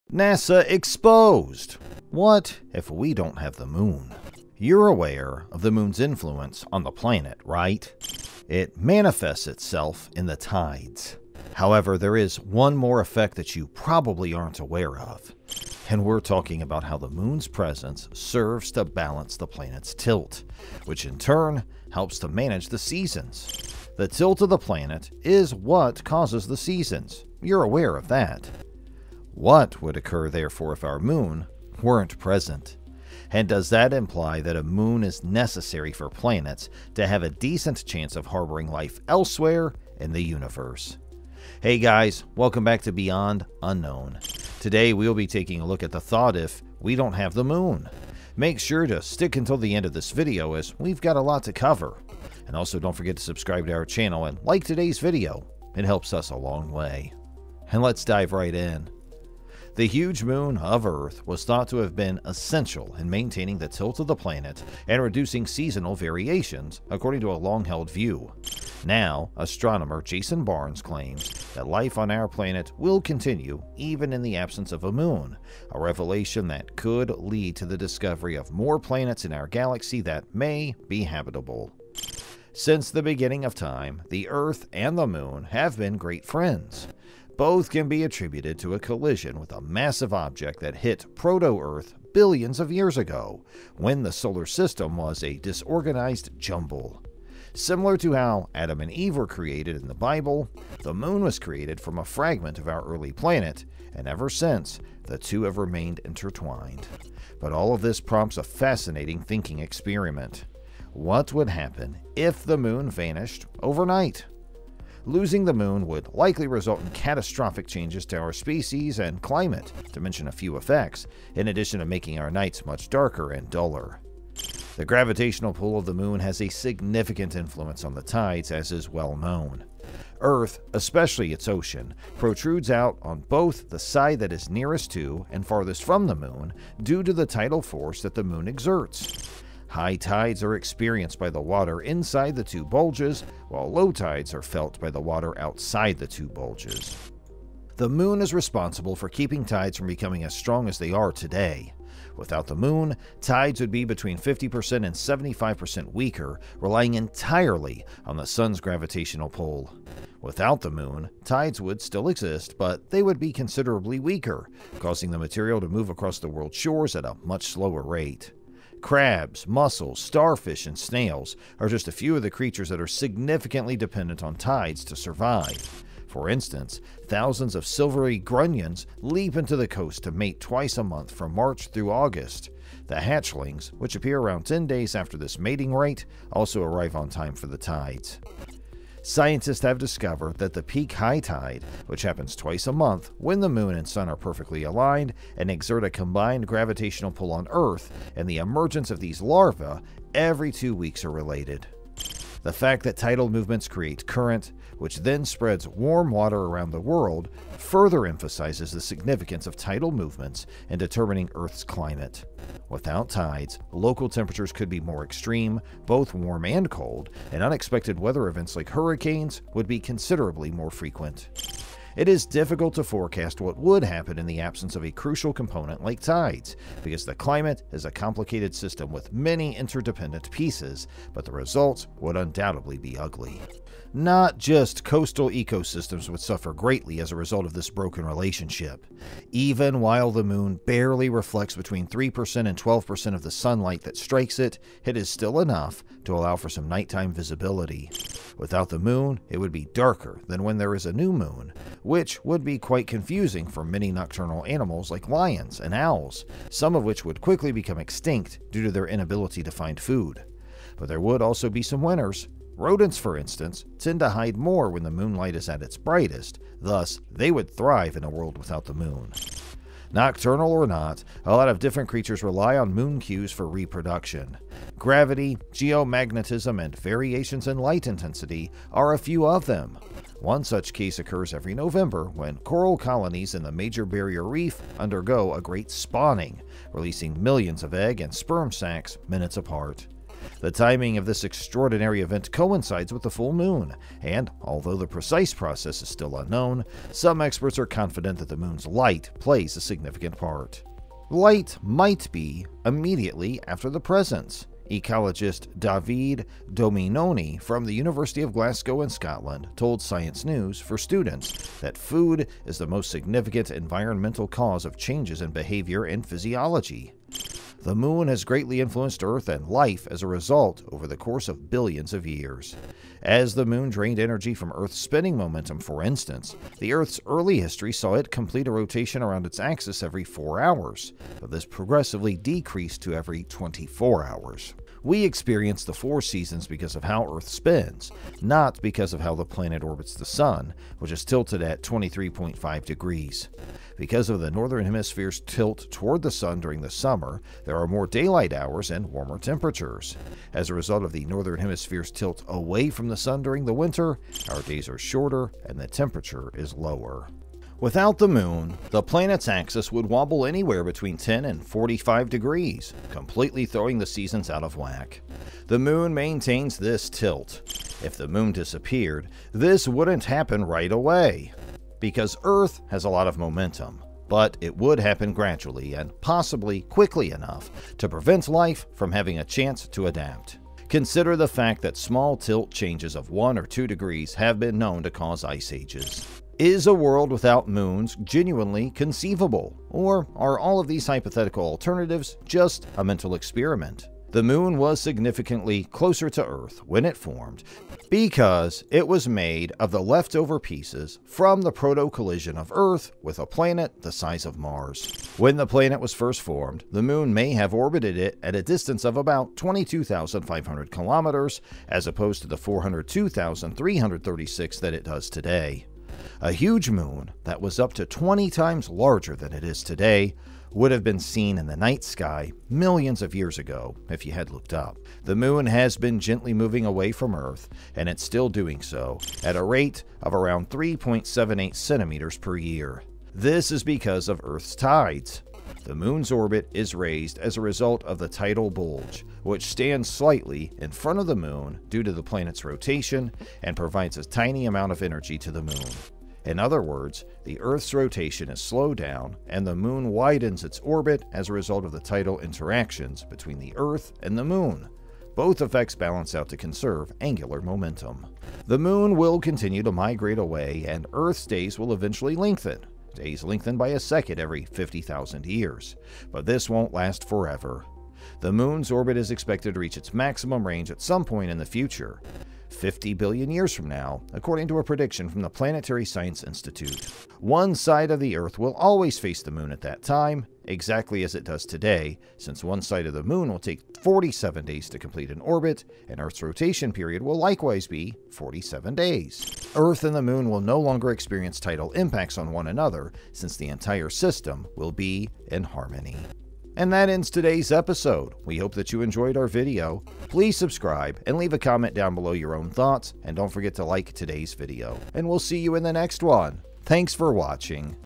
NASA EXPOSED What If We Don’t Have The Moon!
You're aware of the moon's influence on the planet, right? It manifests itself in the tides. However, there is one more effect that you probably aren't aware of. And we're talking about how the moon's presence serves to balance the planet's tilt, which in turn helps to manage the seasons. The tilt of the planet is what causes the seasons. You are aware of that.
What would occur therefore if our moon weren't present? And does that imply that a moon is necessary for planets to have a decent chance of harboring life elsewhere in the universe?

Hi guys! Welcome back to Beyond Unknown. Today we will be taking a look at the thought if we don’t have the Moon. Make sure to stick till the end of this video as we have a lot to cover.
And let’s dive in!

The huge moon of Earth was thought to have been essential in maintaining the tilt of the planet and reducing seasonal variations, according to a long-held view. Now, astronomer Jason Barnes claims that life on our planet will continue even in the absence of a moon, a revelation that could lead to the discovery of more planets in our galaxy that may be habitable.

Since the beginning of time, the Earth and the moon have been great friends. Both can be attributed to a collision with a massive object that hit proto-Earth billions of years ago, when the solar system was a disorganized jumble. Similar to how Adam and Eve were created in the Bible, the moon was created from a fragment of our early planet, and ever since, the two have remained intertwined. But all of this prompts a fascinating thinking experiment: What would happen if the moon vanished over night?

Losing the moon would likely result in catastrophic changes to our species and climate, to mention a few effects, in addition to making our nights much darker and duller.

The gravitational pull of the moon has a significant influence on the tides, as is well known. Earth, especially its ocean, protrudes out on both the side that is nearest to and farthest from the moon due to the tidal force that the moon exerts. High tides are experienced by the water inside the two bulges, while low tides are felt by the water outside the two bulges.

The moon is responsible for keeping tides from becoming as strong as they are today. Without the moon, tides would be between 50% and 75% weaker, relying entirely on the sun's gravitational pull. Without the moon, tides would still exist, but they would be considerably weaker, causing the material to move across the world's shores at a much slower rate. Crabs, mussels, starfish, and snails are just a few of the creatures that are significantly dependent on tides to survive. For instance, thousands of silvery grunions leap onto the coast to mate twice a month from March through August. The hatchlings, which appear around 10 days after this mating rite, also arrive on time for the tides.

Scientists have discovered that the peak high tide, which happens twice a month when the moon and sun are perfectly aligned and exert a combined gravitational pull on Earth, and the emergence of these larvae every two weeks are related.

The fact that tidal movements create current, which then spreads warm water around the world, further emphasizes the significance of tidal movements in determining Earth's climate. Without tides, local temperatures could be more extreme, both warm and cold, and unexpected weather events like hurricanes would be considerably more frequent.

It is difficult to forecast what would happen in the absence of a crucial component like tides because the climate is a complicated system with many interdependent pieces, but the results would undoubtedly be ugly.